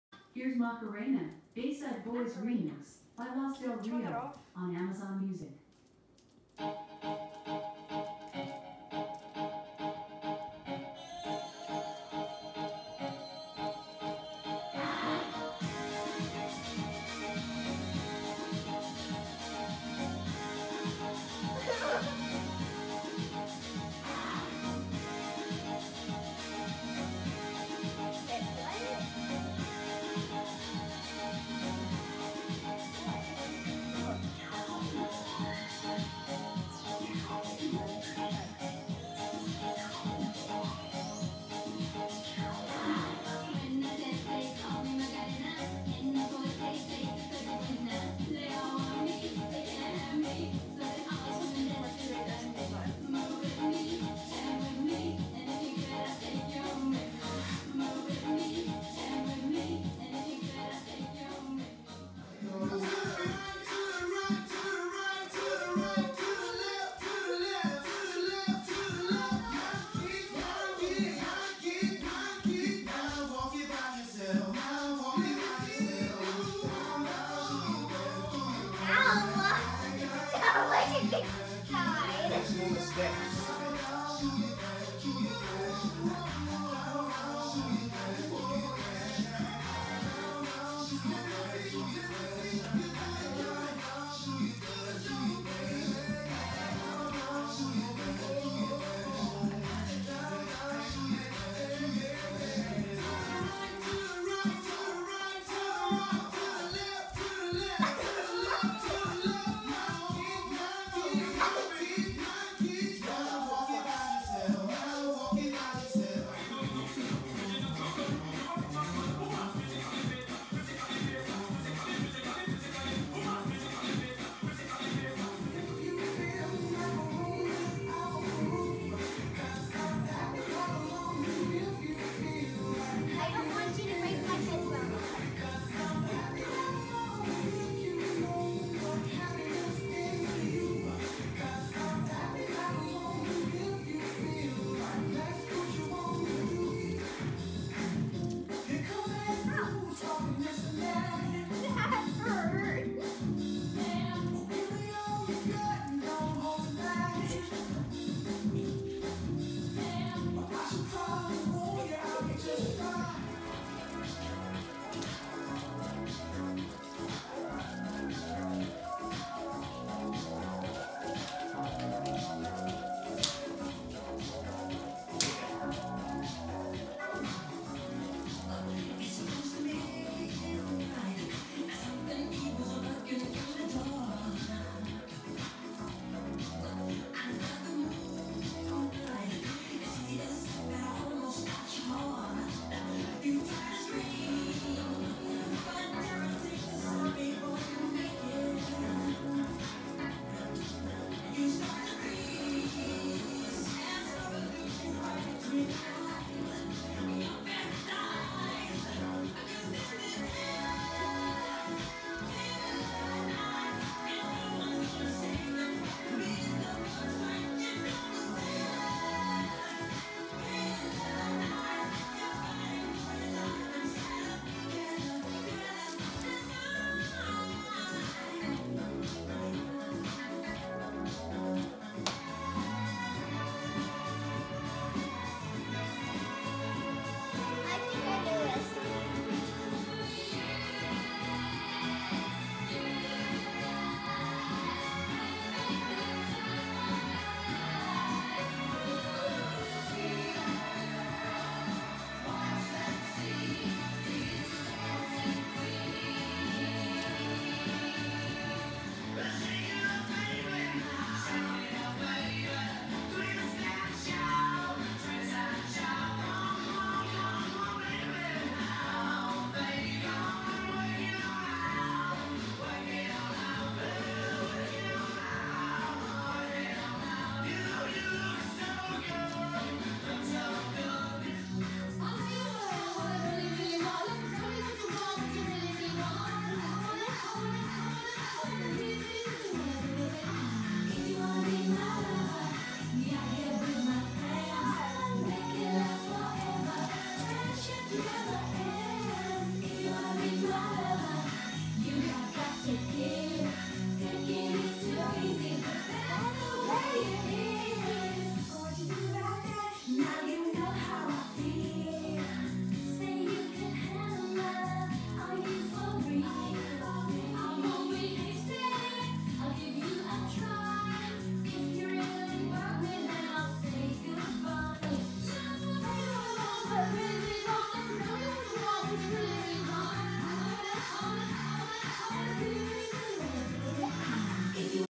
dance party
What a better way to get through quarantine then taping a bunch of glow sticks to your body and dancing with your sister in the dark